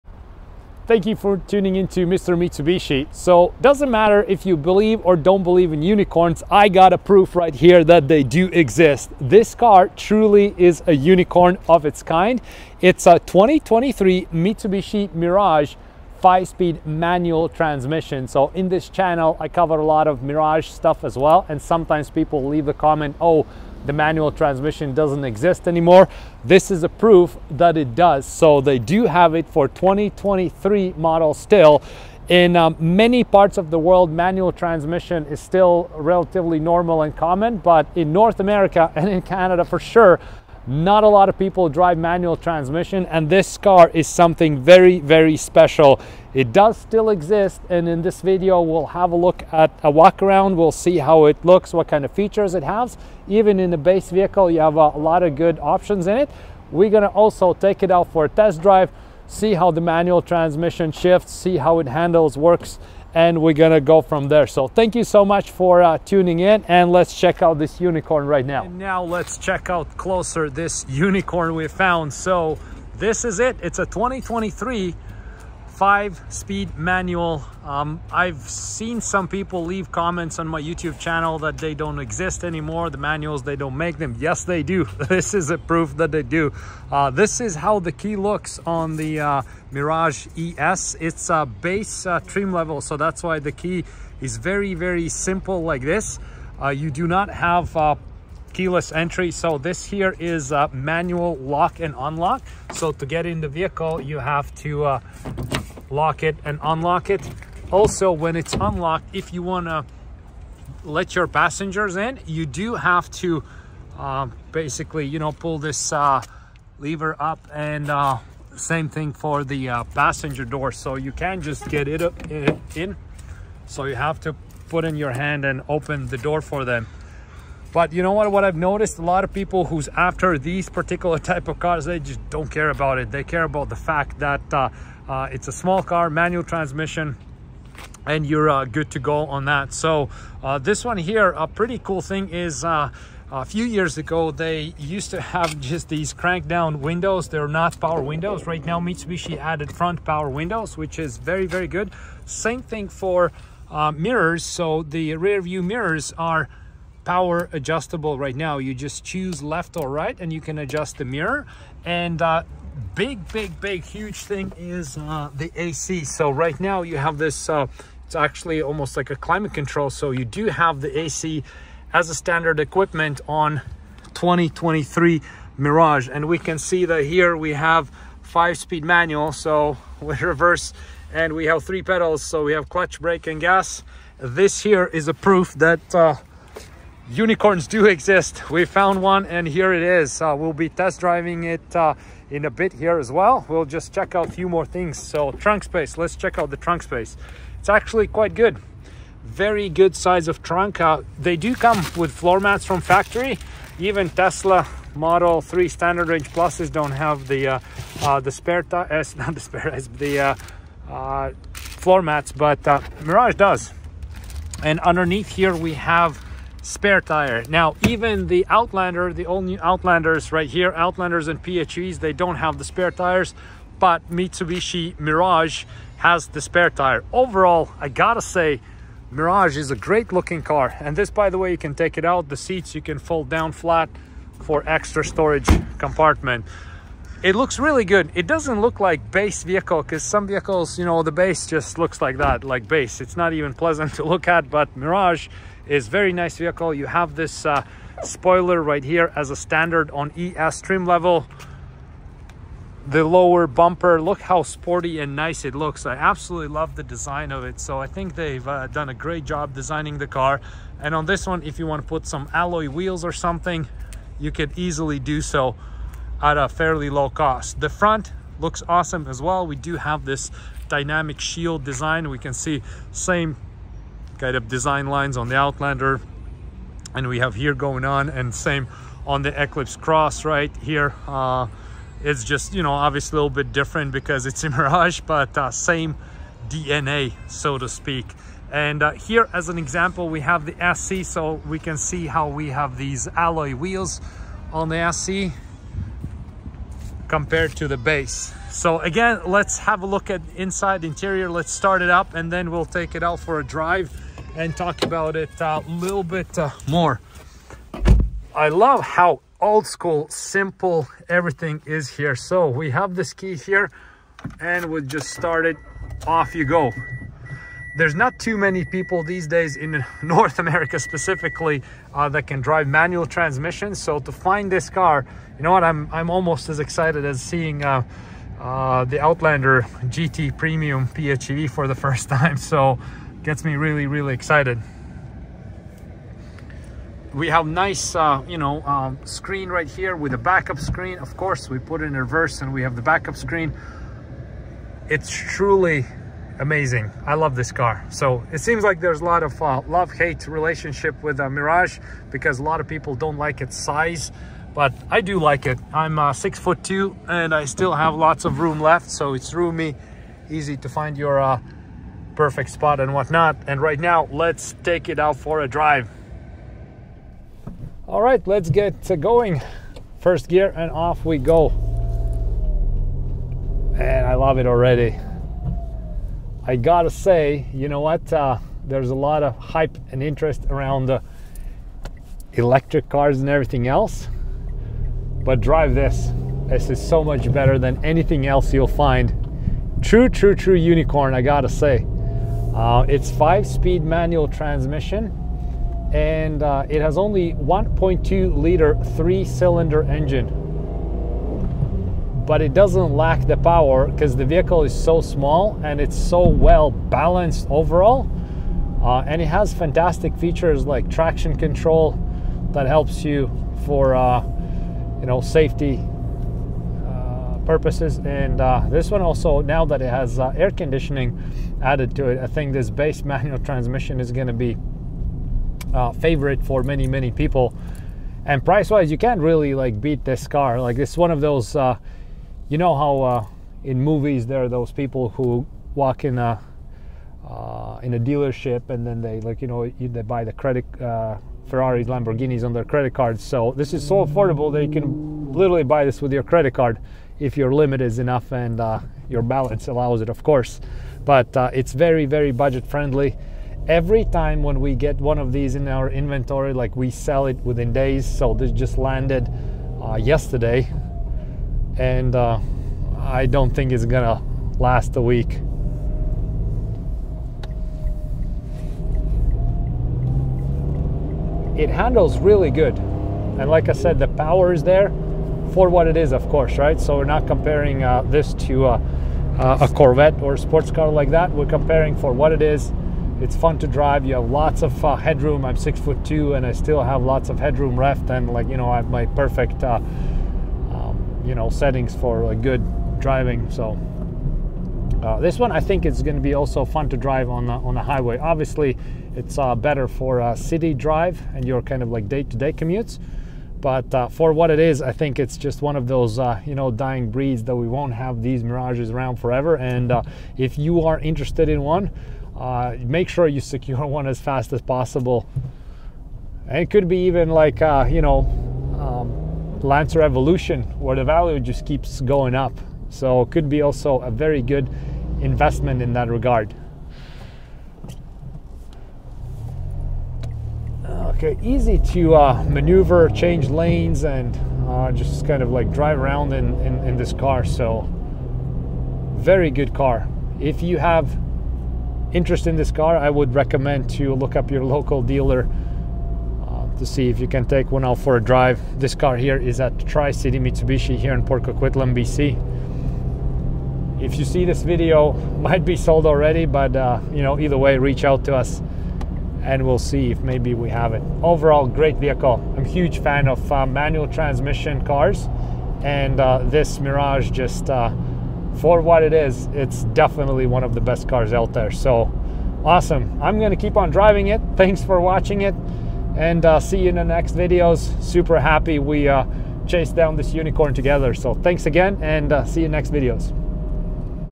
UNICORN - 2023 MANUAL Mirage // Space star!
Mirage or Space Star - 2 names for the smallest Mitsubishi car throughout the world! What's most impressive - it's available in MANUAL transmission!